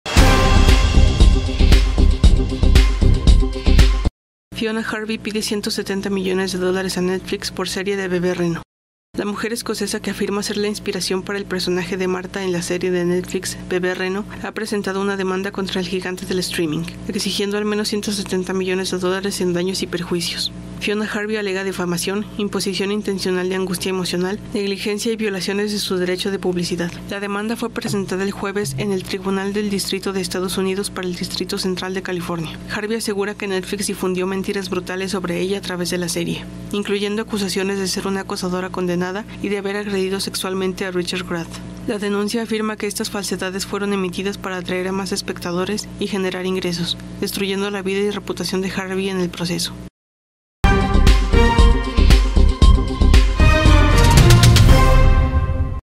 Fiona Harvey pide 170 millones de dólares a Netflix por serie de 'Bebé Reno'
La mujer escocesa que afirma ser la inspiración para el personaje de Martha en la serie de Netflix Bebé Reno, ha presentado una demanda contra el gigante del streaming, exigiendo al menos 170 millones de dólares en daños y perjuicios. Fiona Harvey alega difamación, imposición intencional de angustia emocional, negligencia y violaciones de su derecho de publicidad.

La demanda fue presentada el jueves en el Tribunal de Distrito de Estados Unidos para el Distrito Central de California. Harvey asegura que Netflix difundió "mentiras brutales" sobre ella a través de la serie, incluyendo acusaciones de ser una acosadora condenada y de haber agredido sexualmente a Richard Gadd. La denuncia afirma que estas falsedades fueron emitidas para atraer a más espectadores y generar ingresos, destruyendo la vida y reputación de Harvey en el proceso.